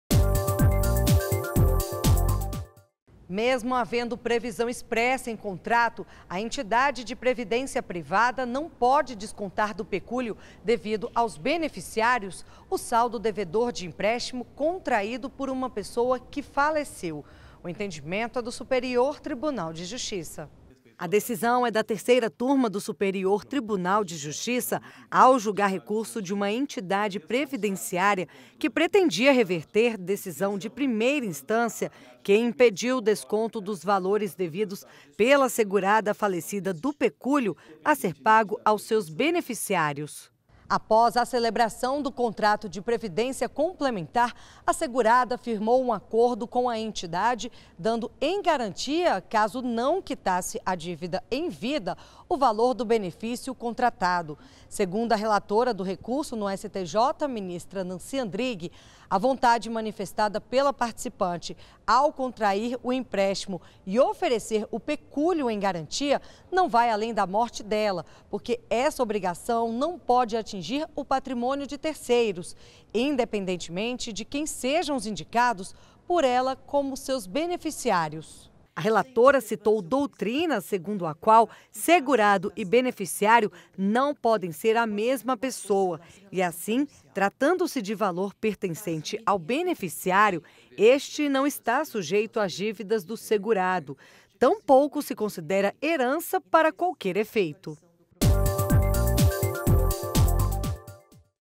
Clique STJ - Empréstimo Previdência (02/01/2019)
Mesmo havendo previsão expressa em contrato, a entidade de previdência privada não pode descontar do pecúlio devido aos beneficiários, o saldo devedor de empréstimo contraído por uma pessoa que faleceu. O entendimento é do Superior Tribunal de Justiça.